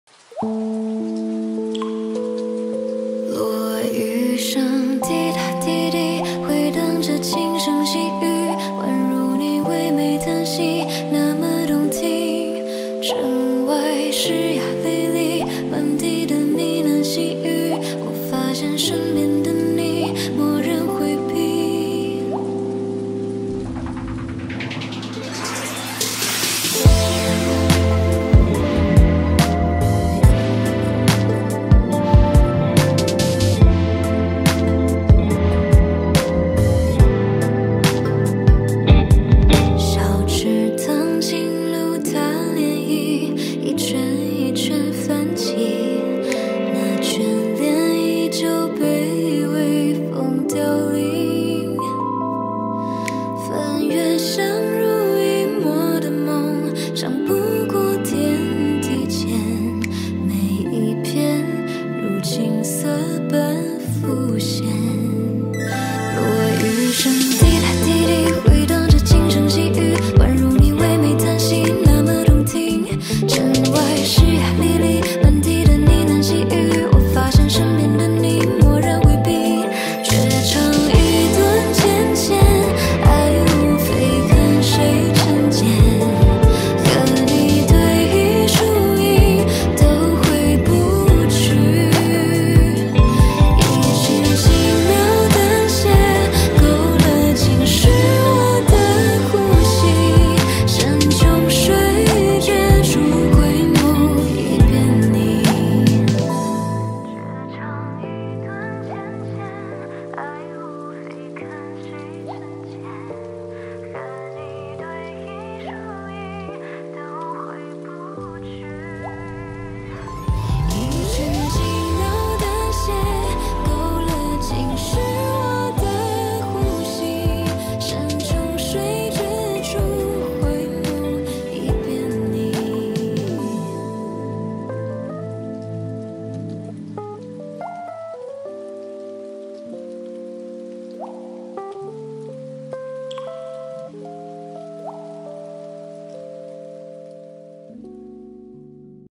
水月镜花 洛神初羡【水下拍摄初尝试】
第一次下水拍摄 本来只是不会游泳 有个摄像机放面前好像突然连憋气都不会了呢…下去之后的表情和不受控制的四肢实在有点一言难尽啊
但为了这么美滴花西子洛神赋礼盒值了！

出品：小仙紫工作室